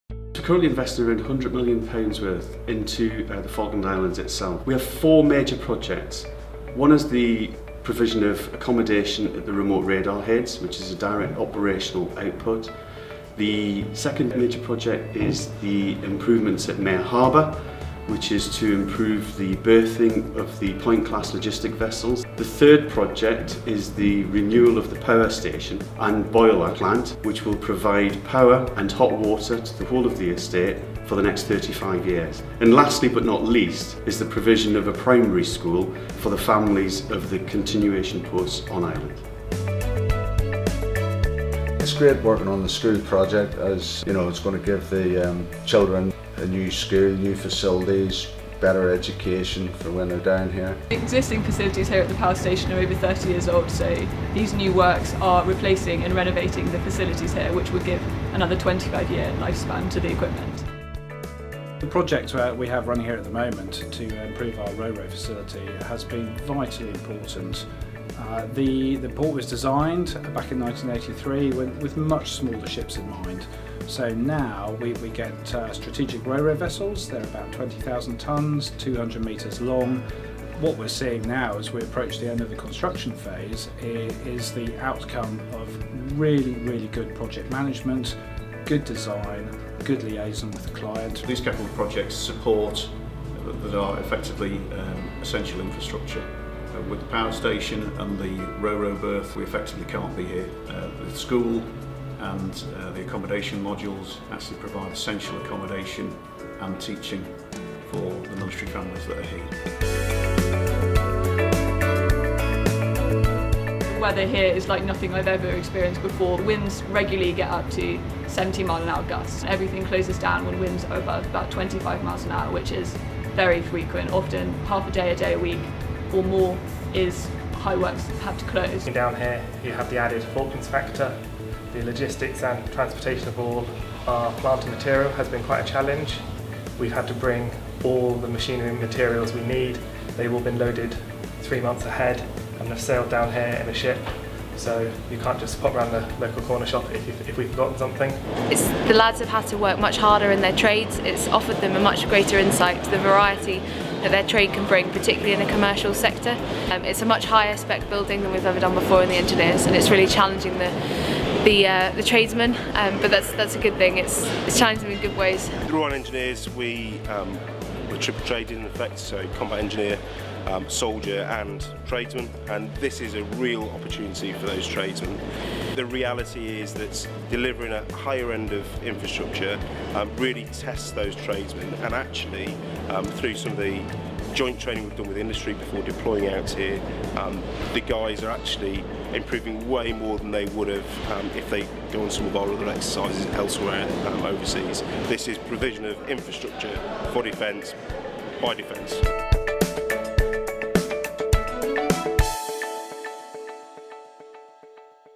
DIO in the Falkland Islands
DIO is currently investing £100m in a series of upgrades to facilities in the Falkland Islands. These include new accommodation at Remote Radar Heads - being undertaken by the Royal Engineers - as well as upgrades to Mare Harbour, a power station and a new school.